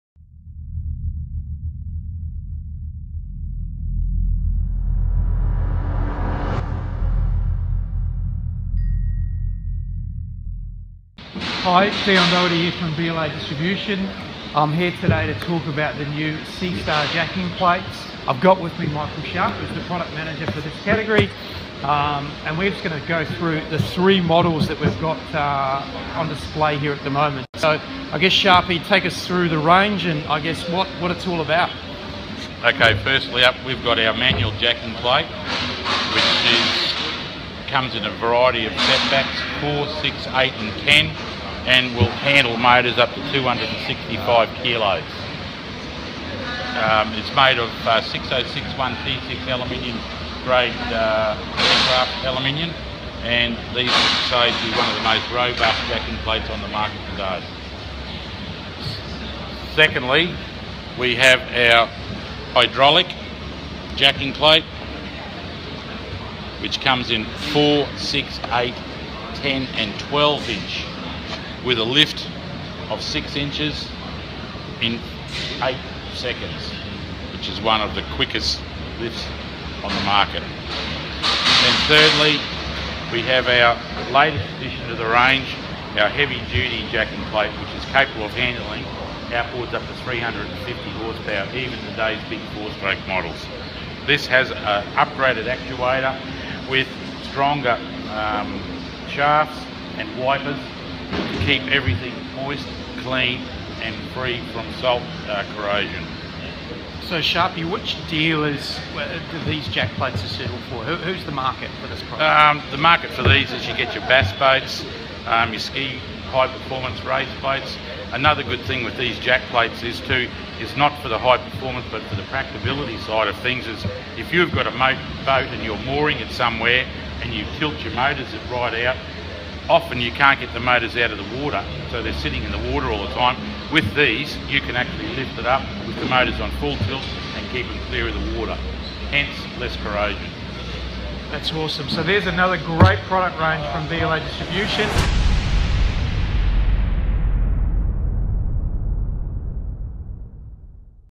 BLA - SeaStar Solutions - JackPlates
LATEST SeaStar hydraulic jackplates from BLA offer heavy duty lifting of outboard engines.

The SeaStar range of manual, hydraulic and heavy duty hydraulic jackplates are one of the sleekest and most durable engine jackplates available.

This essential performance accessory allows for higher speeds, gains in fuel economy and the ability to run in shallow water. Available in 4, 6, 8, 10 and 12 inch setbacks, they feature bold styling that is redefining the look of jackplates.

The new heavy duty models feature an upgraded, high-speed hydraulic actuator. The frames are capable of handling engines up to 350hp that weigh up to 356kg.

“SeaStar Jackplates provide a lift of 6” in 8 seconds, which is one of the quickest on the market,” advises Michael Sharp, brand manager.

“They suit bass boats, and high performance race and ski boats. Another good practical application is for use on moored boats. Often you can’t keep your motors out of the water but with SeaStar Jackplates you can lift your motor up and clear of the water, giving you less corrosion,” adds Sharp.